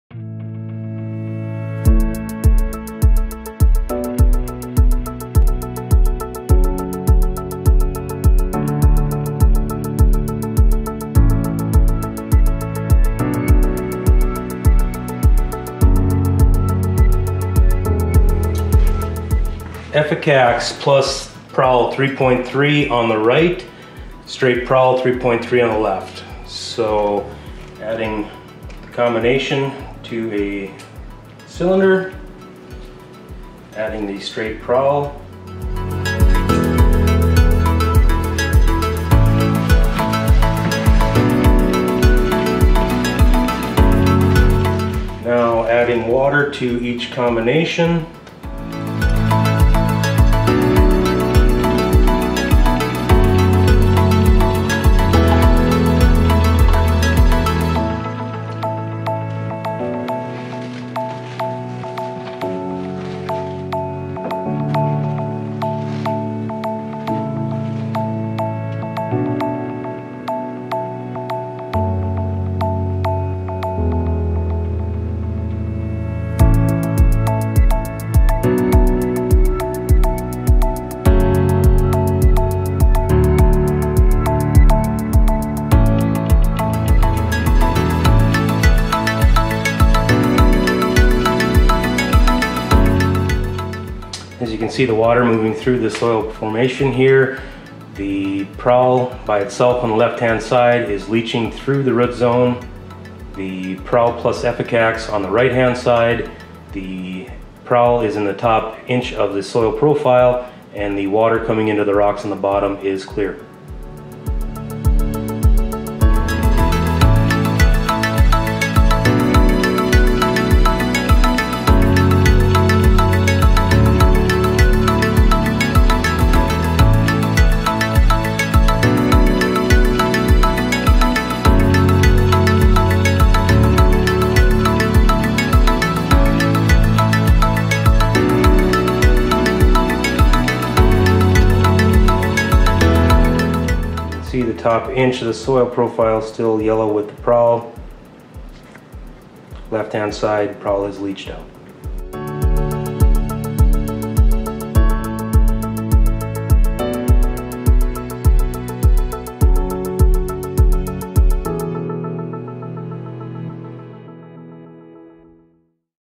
EFFICAX® and PROWL® 3.3 Time Lapse Demonstration
EFFICAX™ is designed to improve the residual activity of most soil-applied herbicides. Utilizing proprietary technology, EFFICAX enhances control of early season weeds by increasing the coverage, absorption and adhesion of spray material deposits onto the soil particles.

This video is a demonstration of what is occurring in the soil after EFFICAX is applied.